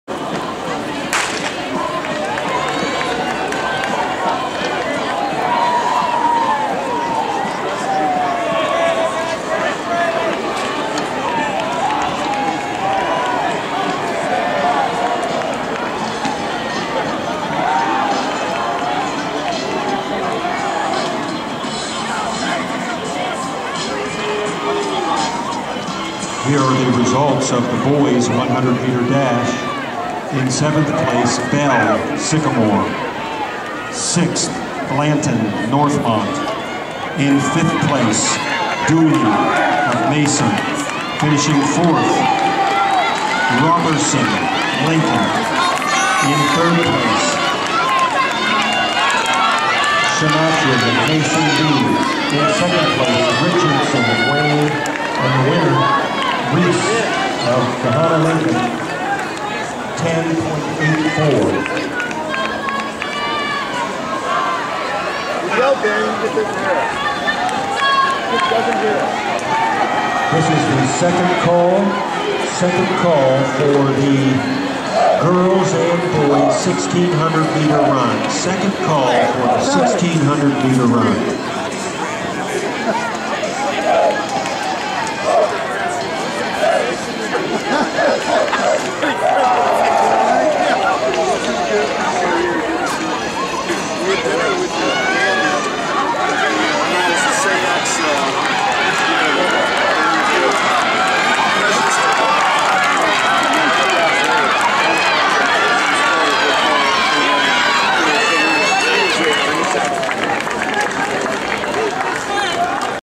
2012 Mason Track Inv - Girls 4x200m Relay (heat 1/2)
Westerville North finished 1st in 1:48.02 minutes and Mason finishing 2nd with 1:49.18 minutes.